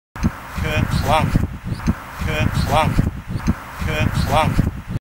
Jonno golf Kerplunk movie
The result of landing a tee shot in the pond.